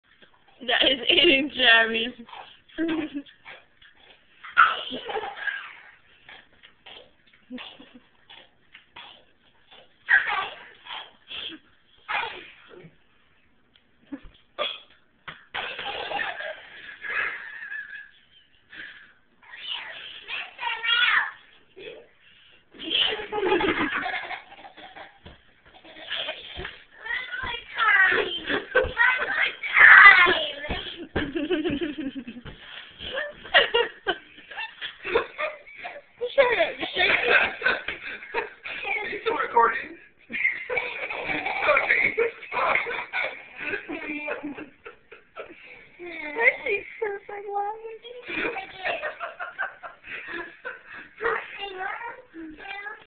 Funny moment with Pajamas
A daddy trying to make his kid laugh by "eating" his kids pj's.